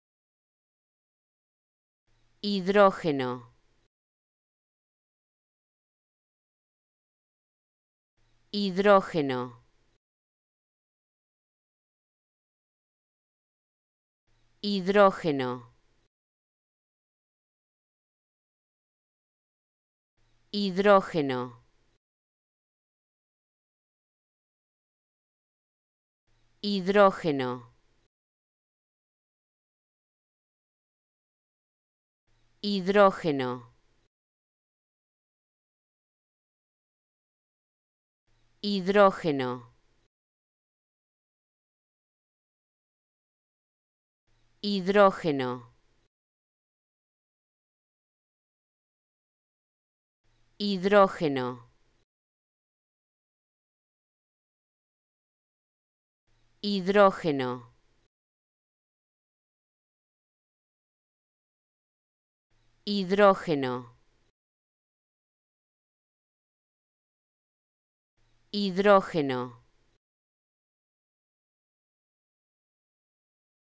Como Pronunciar ''Hidrógeno'' (Hydrogen) Correctamente en Español
Escucha la pronunciación de ''Hidrógeno'' y aprende a decirlo correctamente en Español.